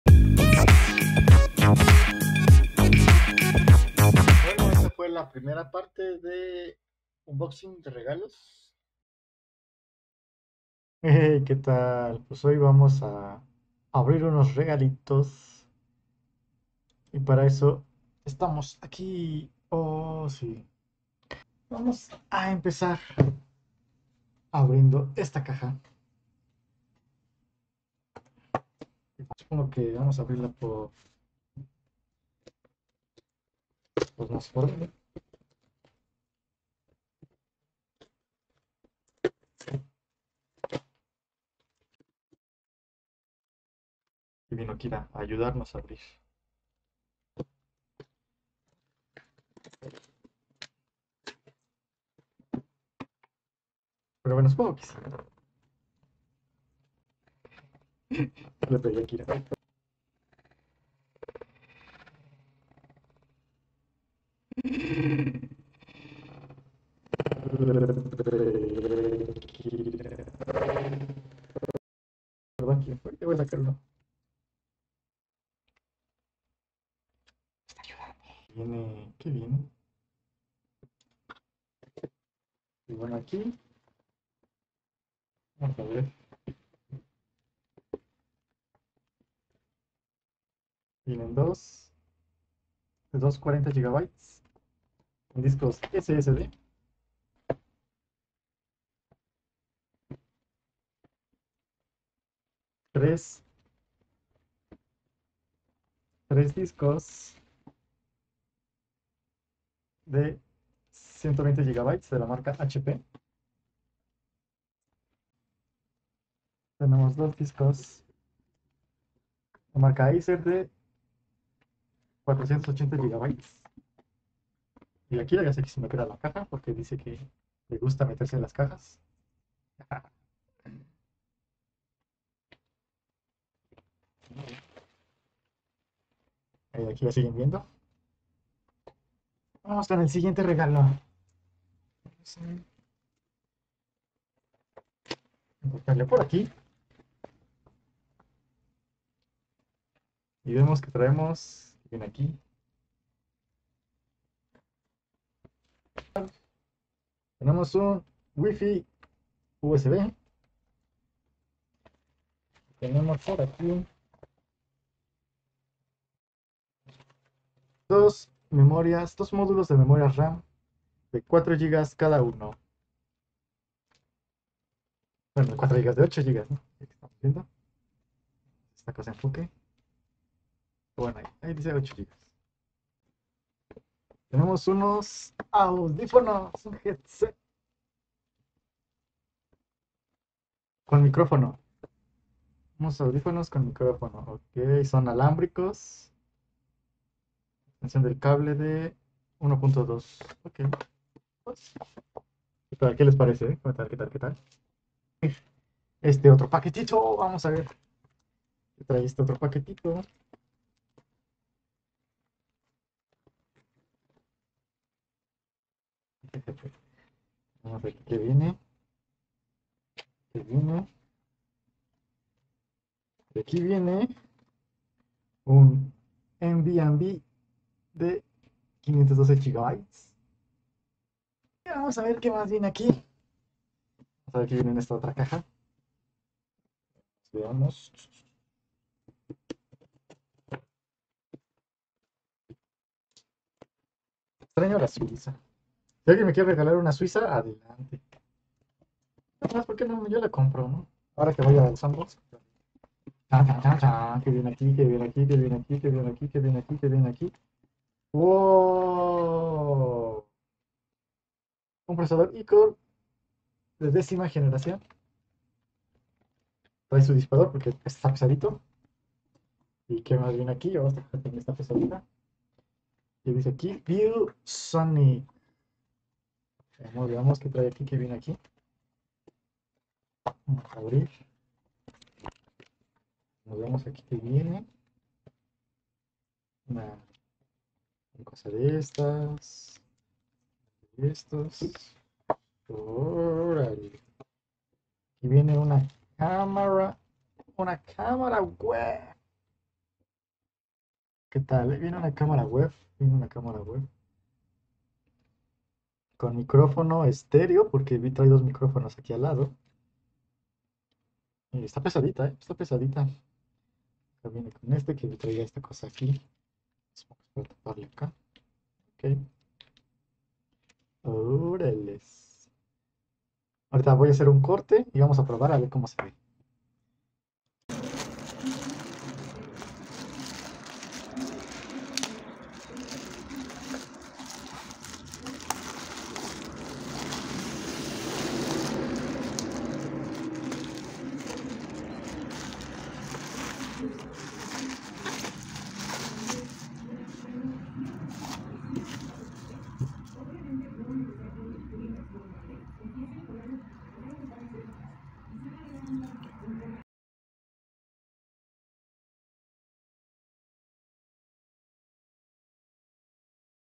Regalos de Cumpleaños 2023 parte 1
Abriendo regalos de suscriptores